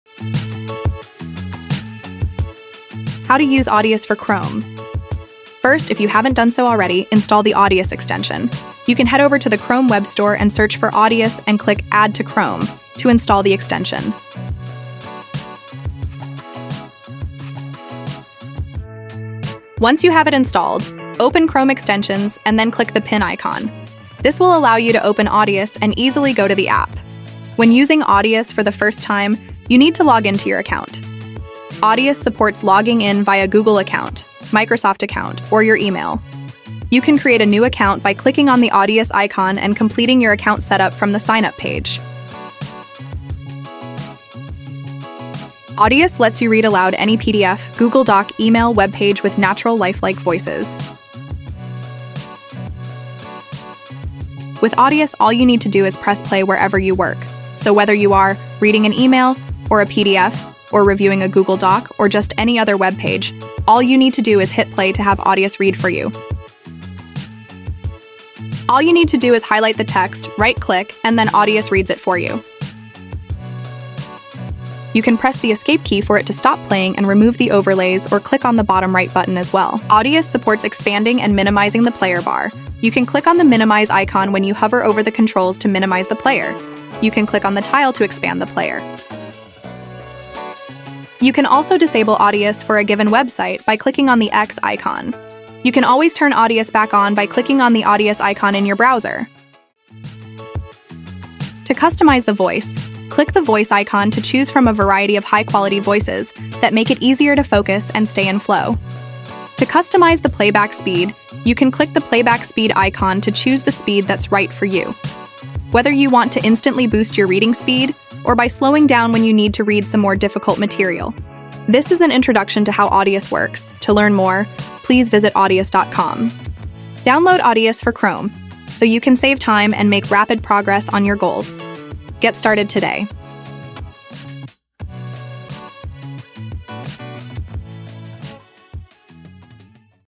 Text to Speech Chrome Extension Guide | How to Use Audeus for Chrome
Welcome to using Audeus Text to Speech for Chrome! This video will introduce you to the various features of Audeus for Chrome, helping you to quickly get started with read aloud for your PDFs, Google Docs, Emails, webpages, & more!

Chapters:
0:00 Introduction
0:03 Download & Install Audeus Text-to-Speech (TTS) Extension for Chrome
0:19 Pin and Open Audeus
0:27 Log in to Audeus
0:43 Overview of Audeus for Chrome - Text to Speech for PDF, Google Docs, & Articles
0:51 How Audeus Text-to-Speech (TTS) Extension Works: Play Button
1:06 How Audeus Text-to-Speech (TTS) Extension Works: Select & Highlight
1:14 How to Exit Highlighter Overlay During Read Aloud
1:19 How to Minimize & Expand the Player
1:29 How to Disable the Player for a Given Website
1:39 How to Customize Your Read Aloud Voice
1:49 How to Customize Your Playback Speed